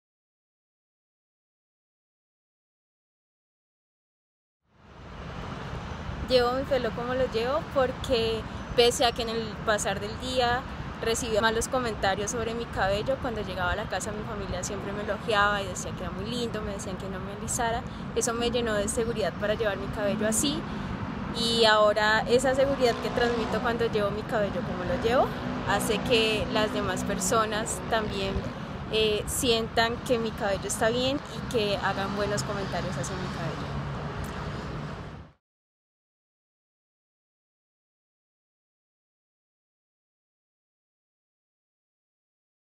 EPT: Ma. Camila Estacio Por qué llevas tu pelo como lo llevas?
Ma. Camila nos cuenta porque lleva su pelo como lo lleva! Si quieres conocer la Anécdota de Pelos de Ma. Camila sigue este enlace